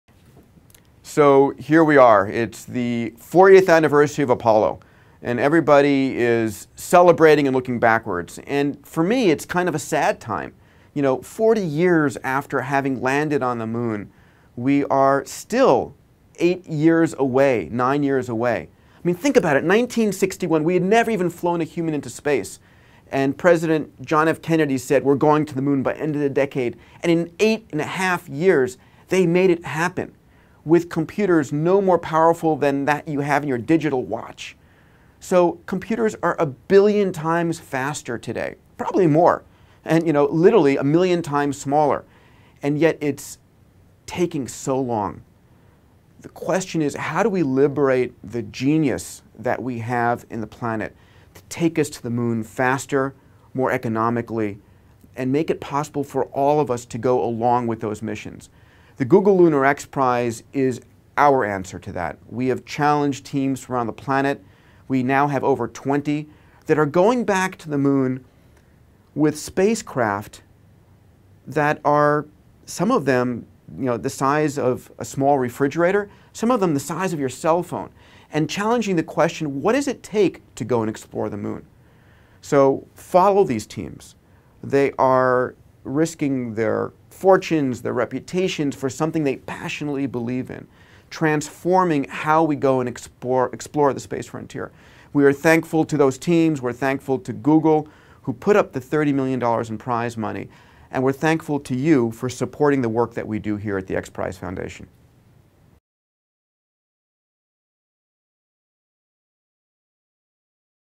40th Anniversary of Apollo 11 Moon Landing
On July 20, 1969, 'Man' walked on the Moon for the first time in history.  As we celebrate the 40th anniversary of that amazing event, Peter Diamandis, Chairman & CEO of the X PRIZE Foundation ponders on what it would take to liberate the genius of our planet to take us back to the Moon, faster and more economically.